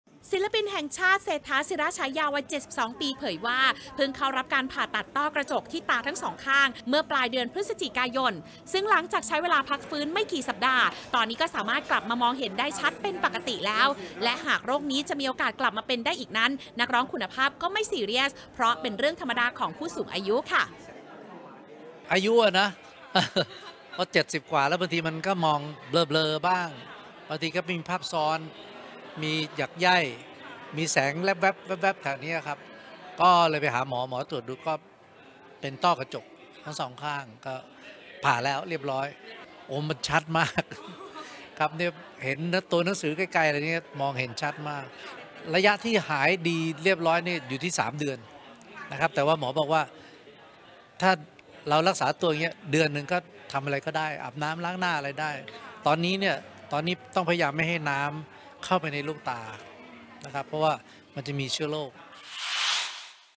"ต้อย" ผ่าตัดต้อกระจก หวังมองเห็นชัดขึ้น
ศิลปินแห่งชาติ “เศรษฐา ศิระฉายา” วัย 72 ปี เผยว่าเพิ่งเข้ารับการผ่าตัดต้อกระจกที่ตาทั้งสองข้างมาเมื่อปลายเดือนพฤศจิกายน ซึ่งหลังจากใช้เวลาพักฟื้นไม่กี่สัปดาห์ ตอนนี้ก็สามารถกลับมามองเห็นได้ชัดเป็นปกติแล้ว และหากโรคนี้จะมีโอกาสกลับมาเป็นได้อีกนั้น นักร้องคุณภาพก็ไม่ซีเรียสเพราะก็เป็นเรื่องธรรมดาของผู้สูงอายุ